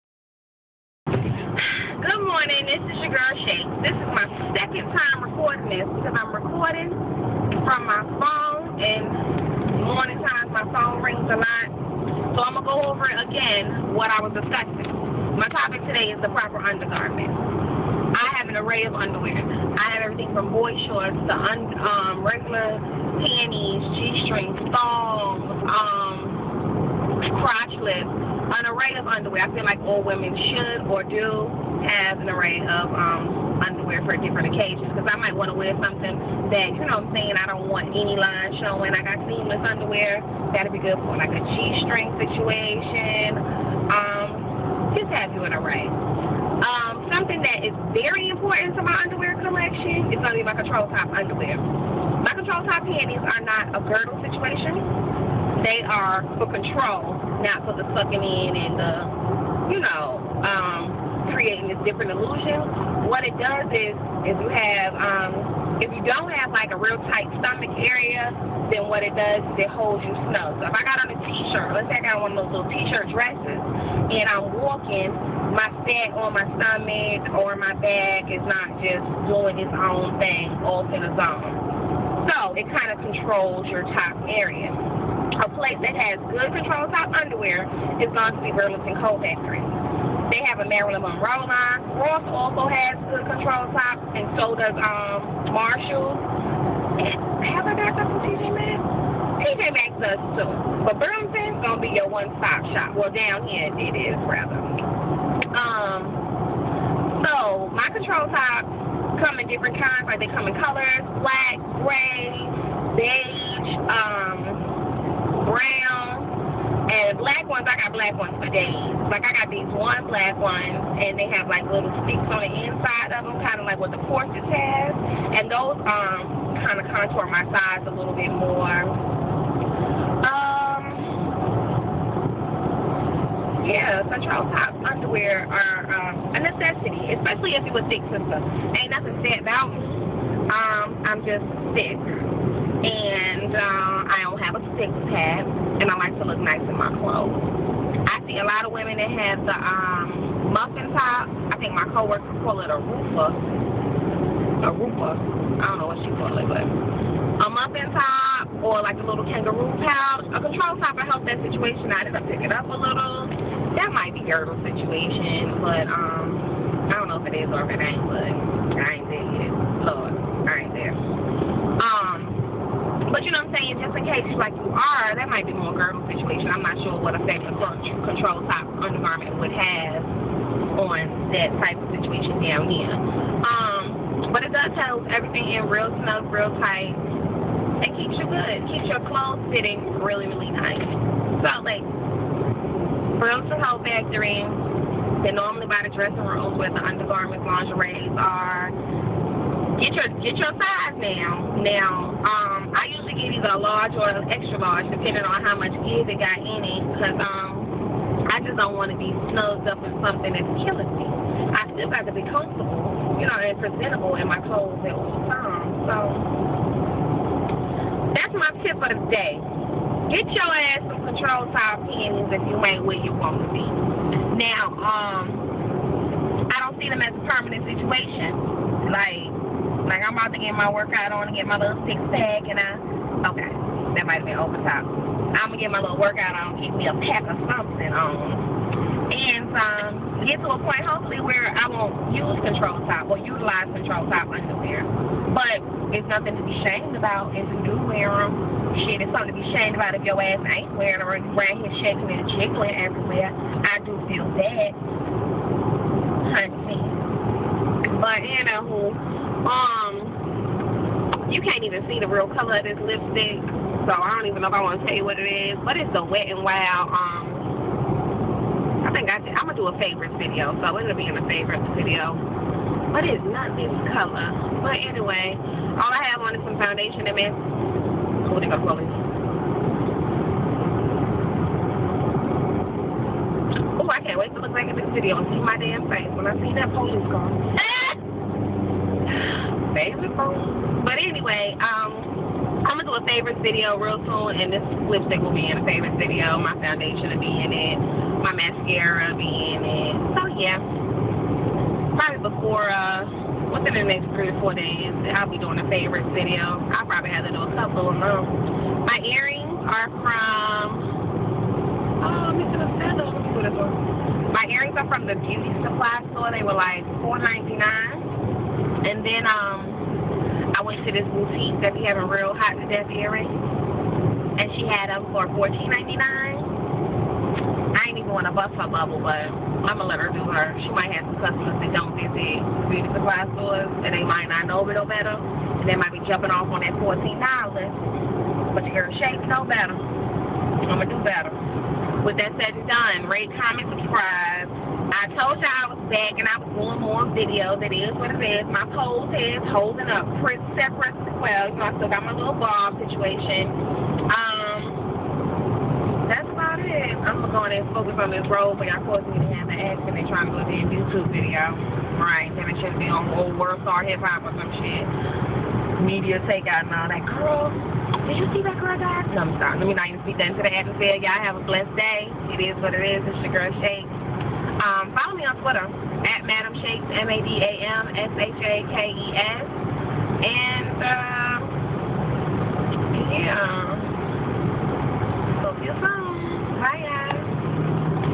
The Proper Under Garments for Plus Size Women
Just a brief chatting with shakes about the proper underwear. Just my opinion!